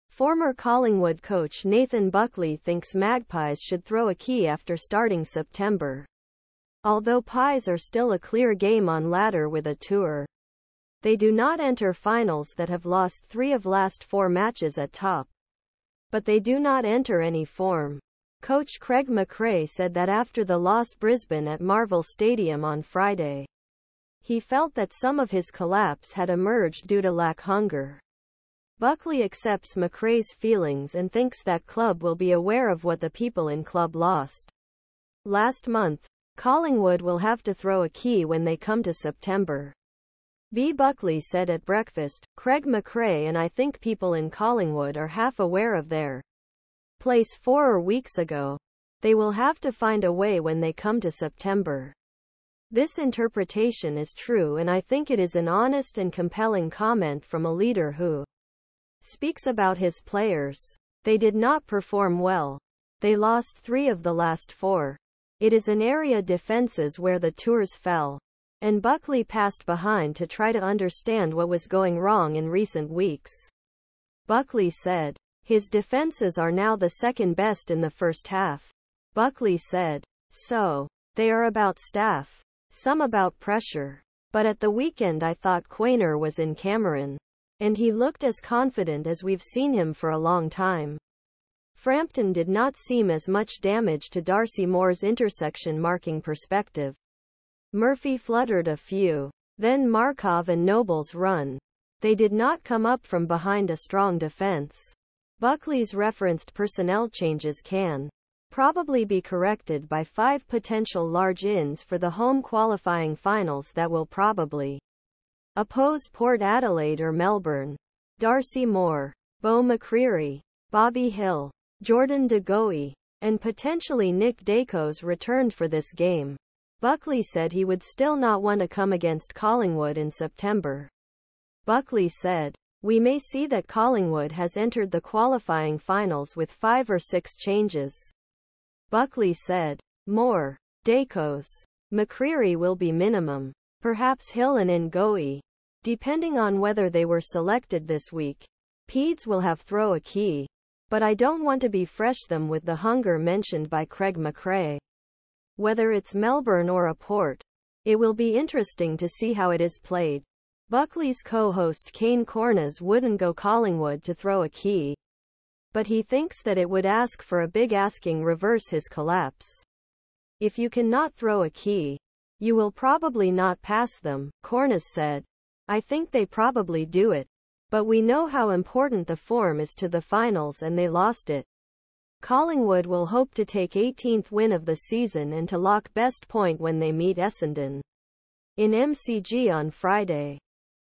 Buckley believes Collingwood must “flick a switch” to fire in September
Former Collingwood coach Nathan Buckley thinks Magpies should throw a key after starting September. Although pies are still a clear game on ladder with a tour, they do not enter finals that have lost three of last four matches at top, but they do not enter any form. Coach Craig Mcrae said that after the loss Brisbane at Marvel Stadium on Friday, he felt that some of his collapse had emerged due to lack hunger. Buckley accepts McRAE's feelings and thinks that club will be aware of what the people in club lost last month. “Collingwood will have to throw a key when they come to September, B Buckley said at Breakfast. “Craig McRAE and I think people in Collingwood are half aware of their place four or weeks ago. They will have to find a way when they come to September. “This interpretation is true and I think it is an honest and compelling comment from a leader who speaks about his players. “They did not perform well. They lost three of the last four." It is an area defenses where the tours fell, and Buckley passed behind to try to understand what was going wrong in recent weeks. Buckley said, "His defenses are now the second best in the first half," Buckley said. “So, they are about staff, some about pressure. “But at the weekend I thought Quaynor was in Cameron, and he looked as confident as we've seen him for a long time. “ Frampton did not seem as much damage to Darcy Moore's intersection marking perspective, Murphy fluttered a few. “Then Markov and Noble's run, they did not come up from behind a strong defense.”  Buckley's referenced personnel changes can probably be corrected by five potential large Ins for the home qualifying finals that will probably oppose Port Adelaide or Melbourne. Darcy Moore, Beau McCreery, Bobby Hill, Jordan de Goey, and Potentially Nick Daicos returned for this game, Buckley said he would still not want to come against Collingwood in September. Buckley said, "We may see that Collingwood has entered the qualifying finals with five or six changes," Buckley said. “Moore, Daicos, McCreery will be minimum, perhaps Hill and in Goey, depending on whether they were selected this week. “Pedes will have throw a key, but I don't want to be fresh them with the hunger mentioned by Craig McCrae. "Whether it's melbourne or a port, it will be interesting to see how it is played." Buckley's co -host Kane Cornes wouldn't go Collingwood to throw a key, but he thinks that it would ask for a big asking reverse his collapse. “If you can not throw a key, you will probably not pass them,” Cornes said. “I think they probably do it. But we know how important the form is to the finals and they lost it." Collingwood will hope to take 18th win of the season and to lock best point when they meet Essendon in McG on Friday.